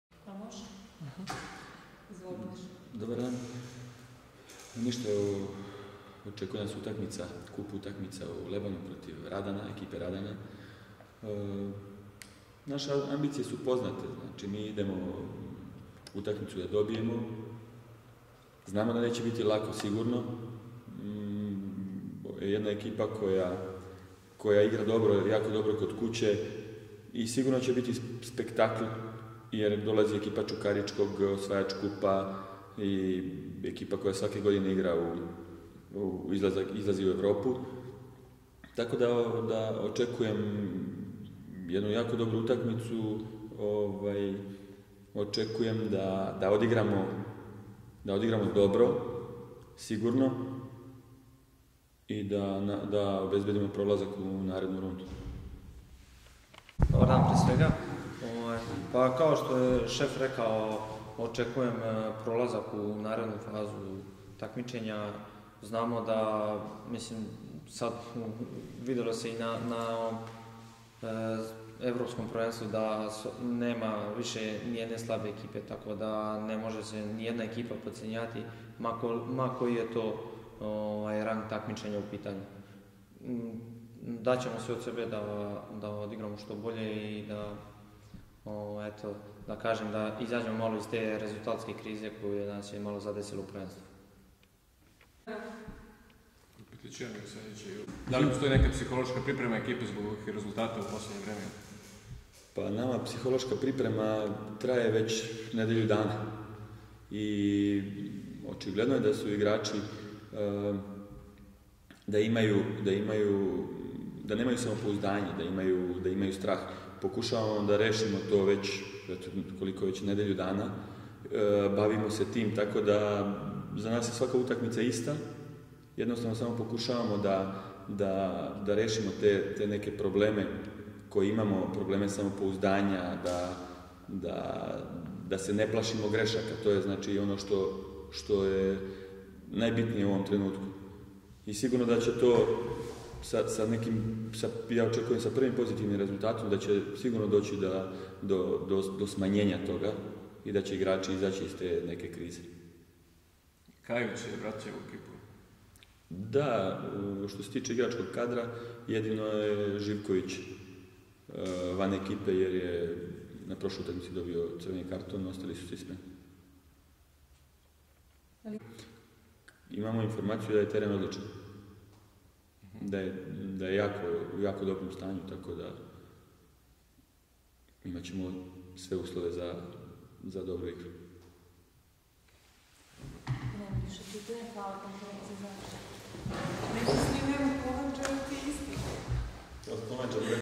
Čukarički u Lebanima, Petrić na brdu
Fudbaleri Čukaričkog dobili su novog trenera - na klupu "brđana" sešće nekadašnji as Partizana Gordan Petrić, ali tek posle utakmice šesnaestine finala Kupa protiv Radana. O gostovanju u Lebanima na konferenciji za medije govorio je Nenad Mirosavljević